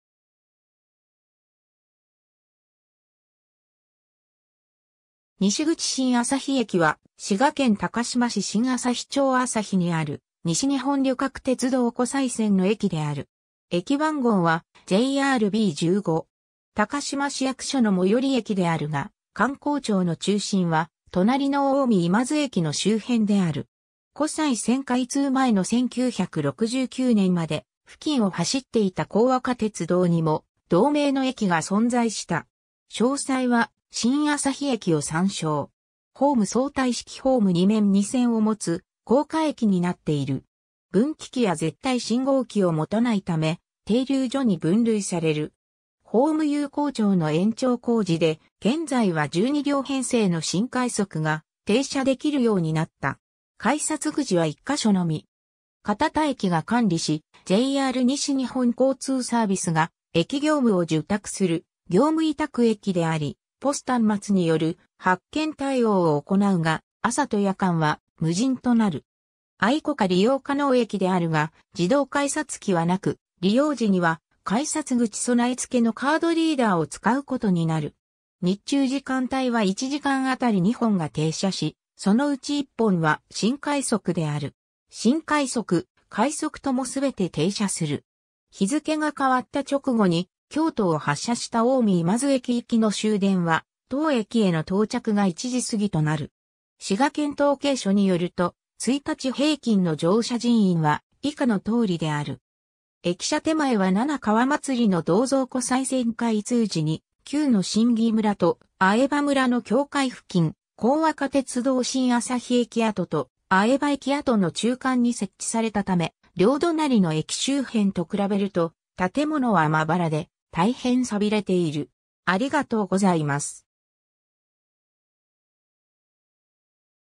新旭駅
西口 新旭駅（しんあさひえき）は、滋賀県高島市新旭町旭にある、西日本旅客鉄道（JR西日本）湖西線の駅である。
駅番号はJR-B15。
高島市役所の最寄り駅であるが、官公庁の中心は隣の近江今津駅の周辺である。
 湖西線開通前の1969年まで付近を走っていた江若鉄道にも、同名の駅が存在した。
詳細は新旭駅 (江若鉄道) を参照。
 ホーム 相対式ホーム2面2線を持つ高架駅になっている。
ホーム有効長の延長工事で、現在は12両編成の新快速が停車できるようになった（以前は新快速停車駅では唯一12両編成が停車できなかった）。
改札口は1か所のみ。
 堅田駅が管理し、JR西日本交通サービスが駅業務を受託する業務委託駅であり、POS端末による発券対応を行うが、朝と夜間は無人となる。
ICOCA利用可能駅（相互利用可能ICカードはICOCAの項を参照）であるが自動改札機はなく、利用時には改札口備え付けのカードリーダーを使うことになる。
 日中時間帯は1時間あたり2本が停車し、そのうち1本は新快速である。
新快速・快速ともすべて停車する。
日付が変わった直後に京都を発車した近江今津駅行きの終電は、当駅への到着が1時過ぎとなる。
 「滋賀県統計書」によると、1日平均の乗車人員は以下の通りである。
 駅舎　手前は七川祭の銅像 湖西線開通時に旧の新儀村と饗庭村の境界付近、江若鉄道新旭駅跡と饗庭駅跡の中間に設置されたため、両隣の駅周辺と比べると建物はまばらで、大変寂れている。